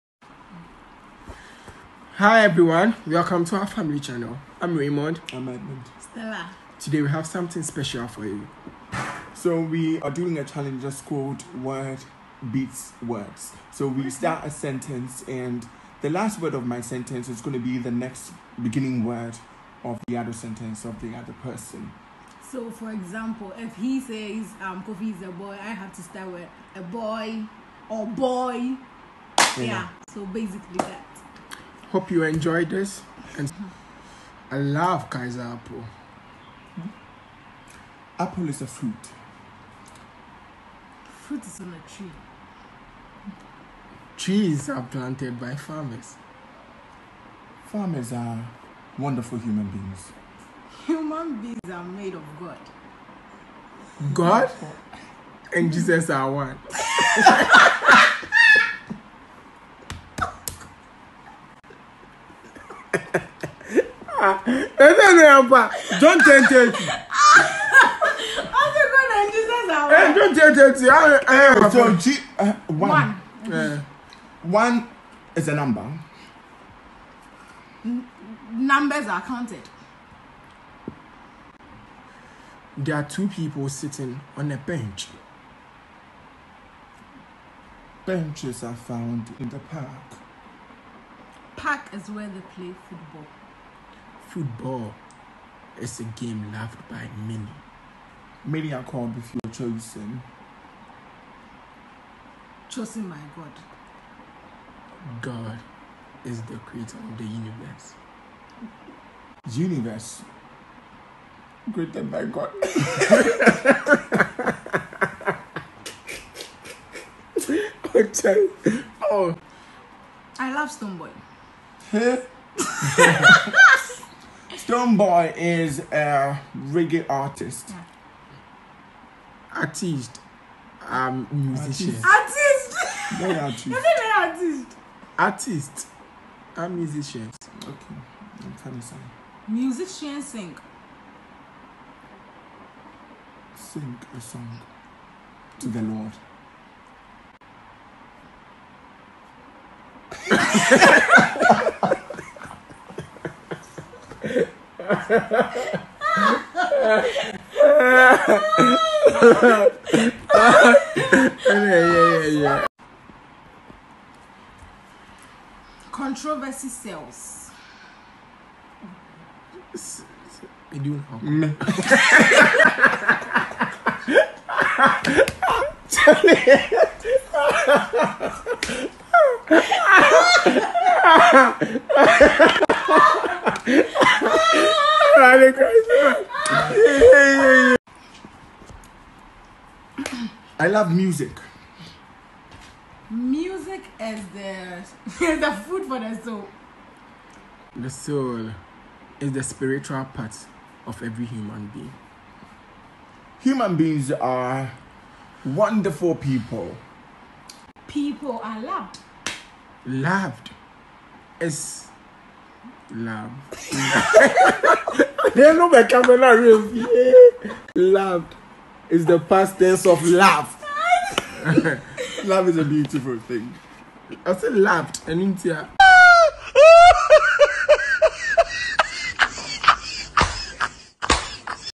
Family Challenge: Word beats Words
This is a game we titled Word beats words.
The last word of a sentence becomes the beginning word of the next sentence from the other person.

We had a lot of fun filming this and we hope this video brings joy and happiness to everyone who gets to see it too.
With Love
Eddy, Ray, & Stella